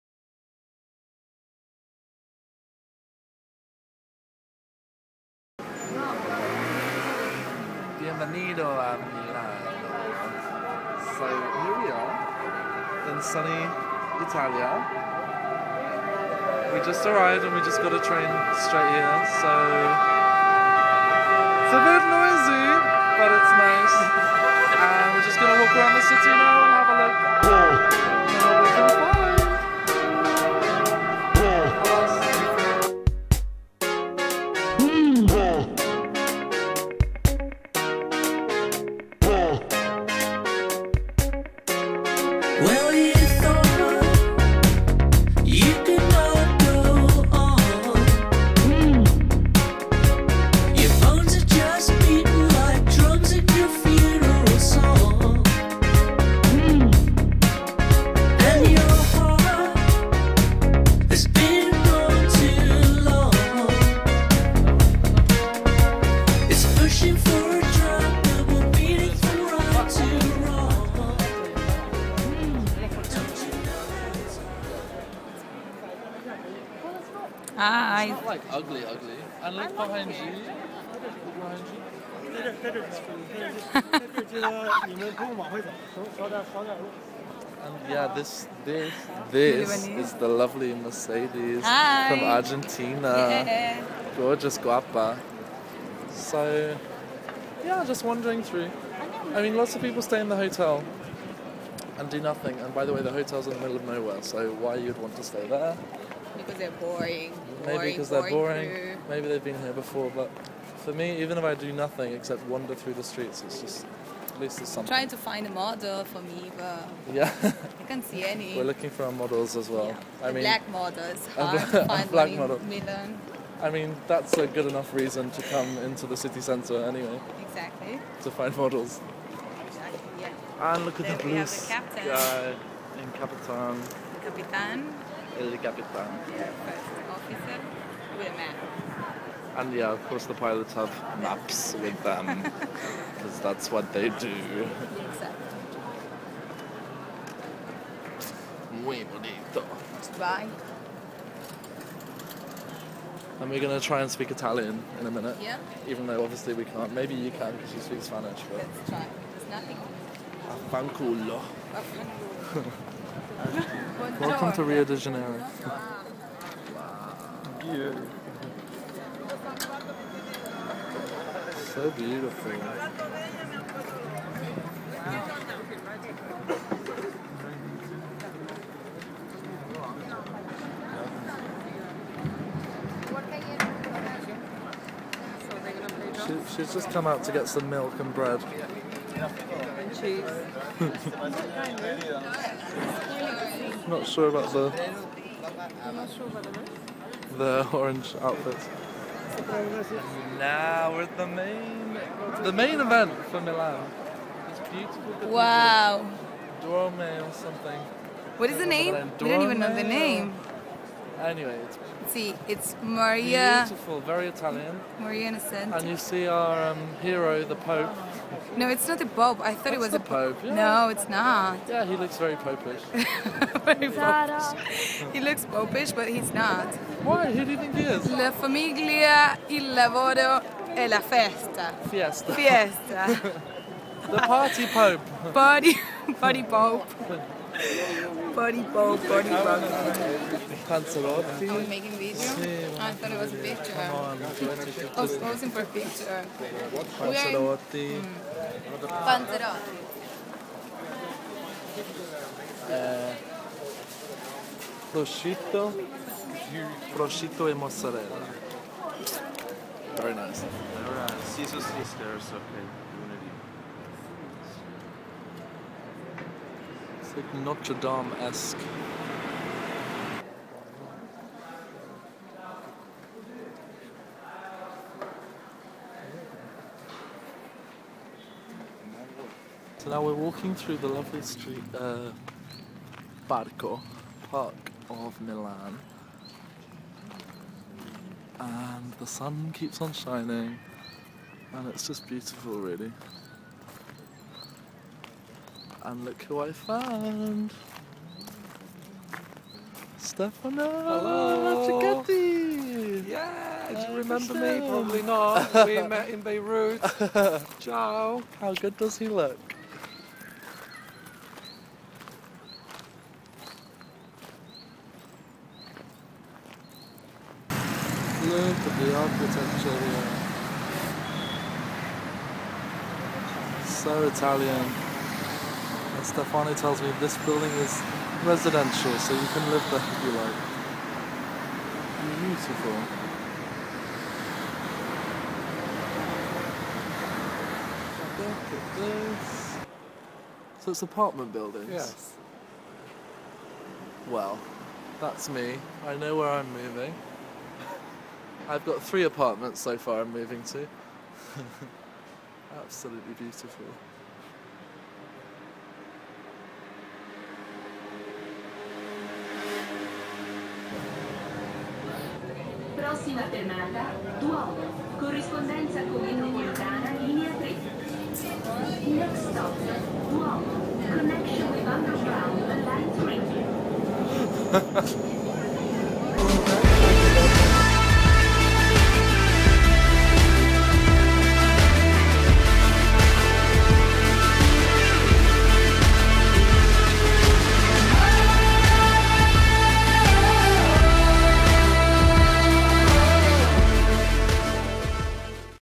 FlyGuy goes to... MILANO with Stefano!
Took a quick plane to Italia, 24 hours later I headed back to Dubai having spent a beautiful day exploring this gem in northern Italy...